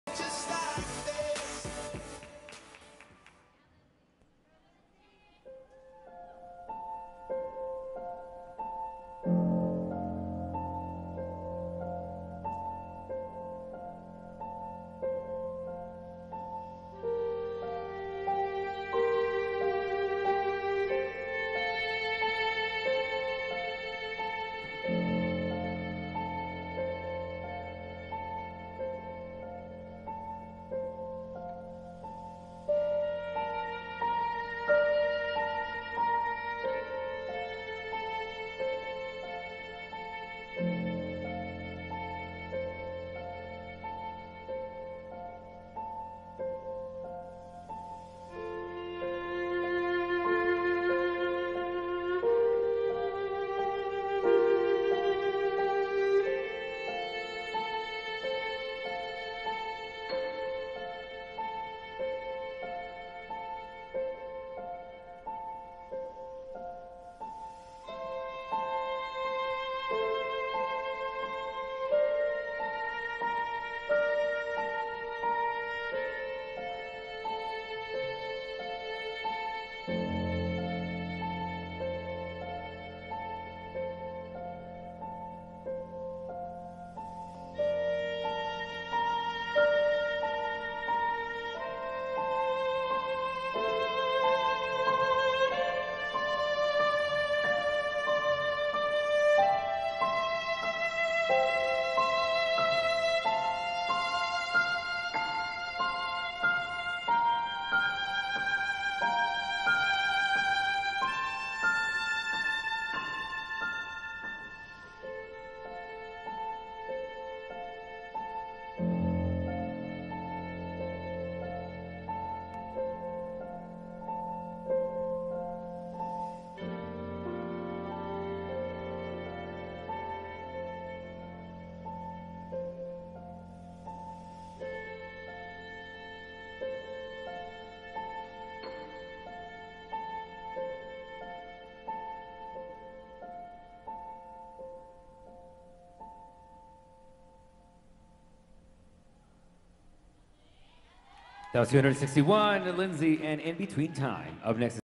Lindsey Wade - In Between Time
Prodigy Dance And Performing Arts Centre
Teen Solo for Best Dancer